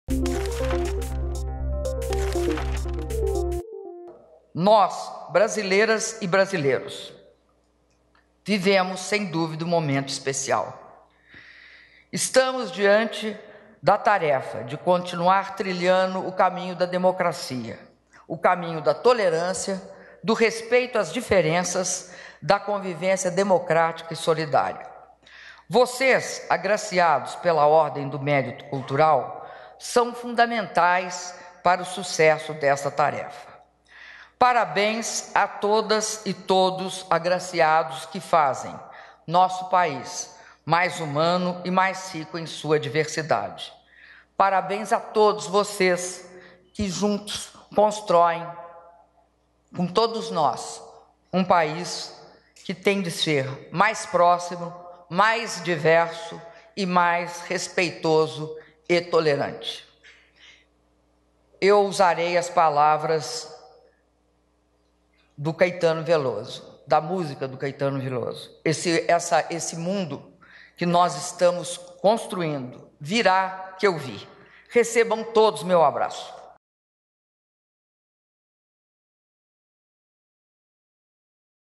Presidenta Dilma ressalta importância das manifestações culturais para a democracia do país
ATIVIDADES DA PRESIDENTA - 09.11.15: Dilma Rousseff voltou a falar sobre a importância da democracia, tolerância e respeito às diferenças durante entrega da Ordem do Mérito Cultural 2015. O evento reuniu, na noite desta segunda-feira, personalidades, grupos ou instituições reconhecidas por suas contribuições culturais ao país.